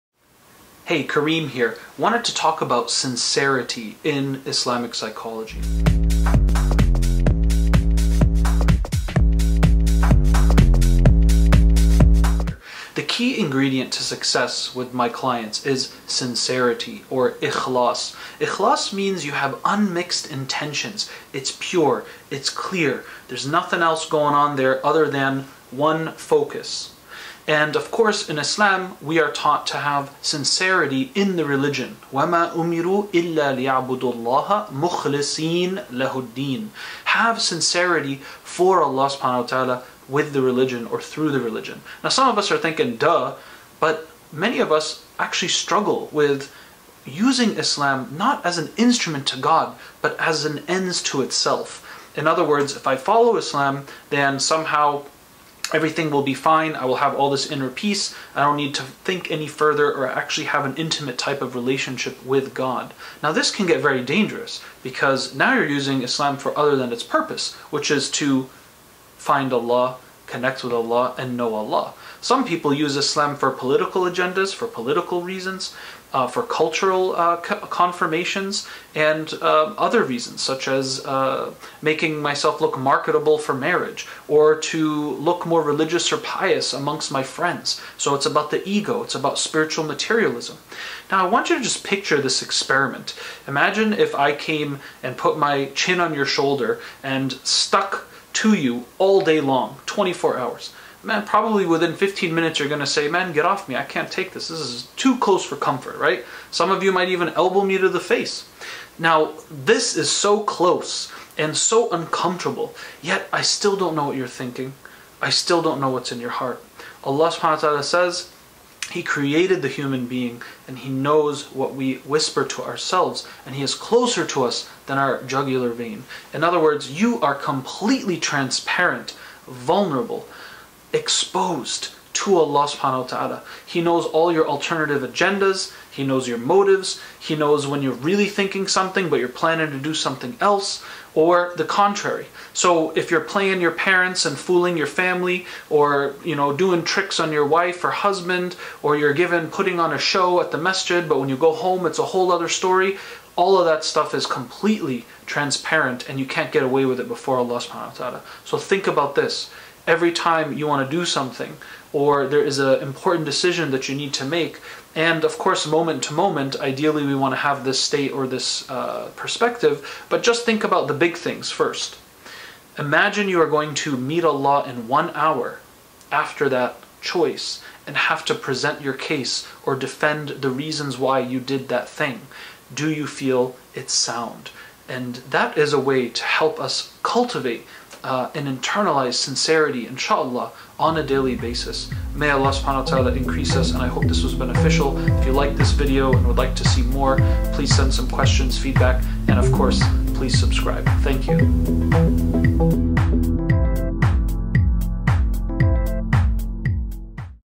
SINCERITY IN ISLAM - Karim Serageldin
What does it mean to be sincere and how can we cultivate the Islamic psychology of sincerity? This tip will help you become more honest and authentic before Allah.